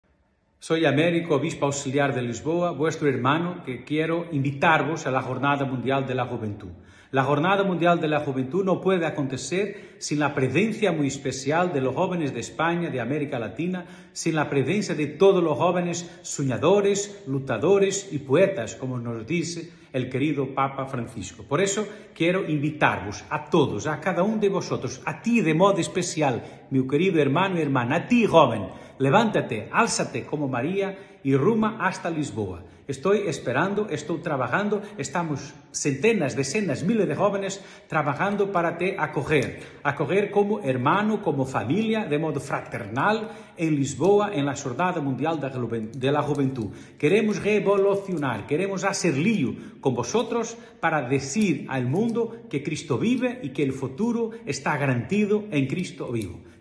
Rezar Hoy - Mensaje de Mons. Américo Aguiar, Obispo Auxiliar-Lisboa, responsable JMJ 2023
Meditaciones y reflexiones para hacer la oración especialmente dirigidas a jóvenes. Mons. Américo Aguiar, Obispo Auxiliar de Lisboa y responsable de la JMJ de 2003 en la capital de Portugal nos invita a ir a la aventura de vivir una JMJ cerca de la casa de la Casa de la Virgen peregrina en Fátima.
¿Has vivido alguna JMJ? Que no te la cuenten, vívela!!!